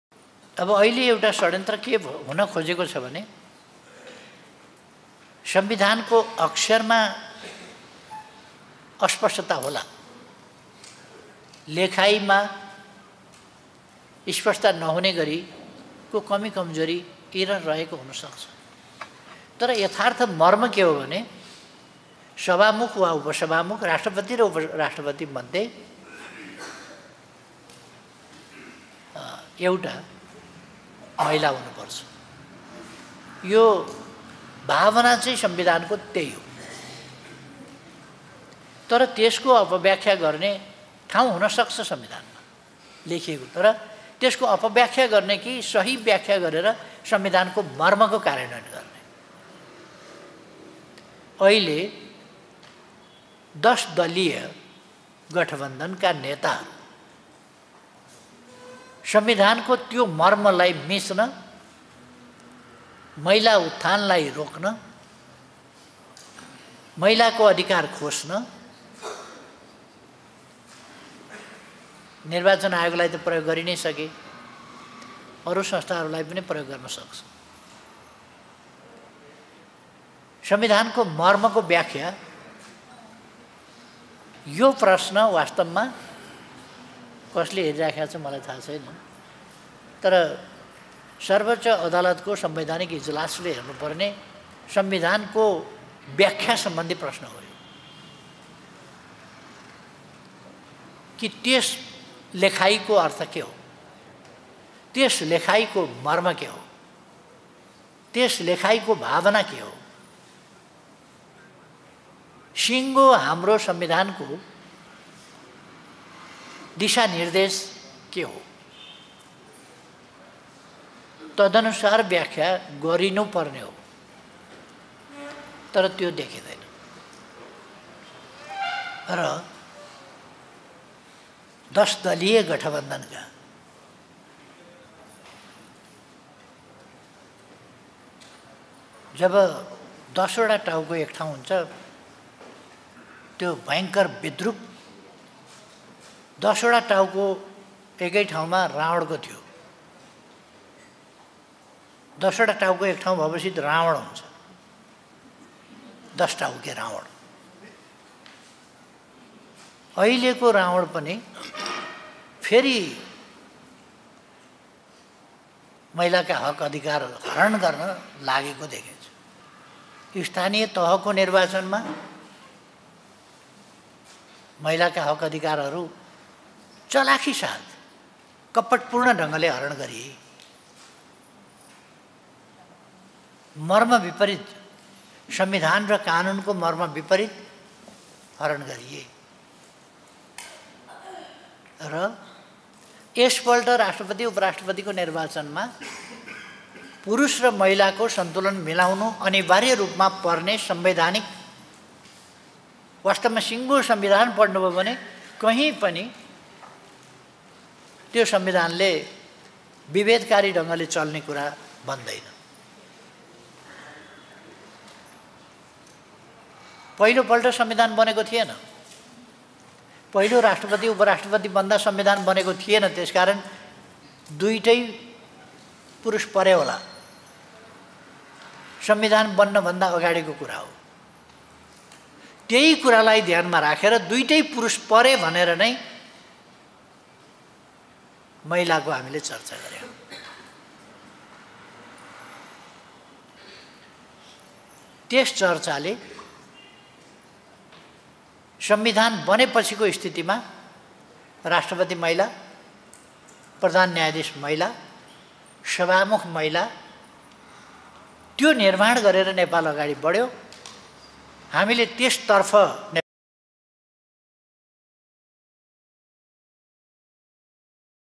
अहिलेको सत्ता गठबन्धन दश टाउके रावण हो, यसले महिलाको अधिकार हरण गर्न खोज्दैछ: केपी ओली
Source Video: Setopati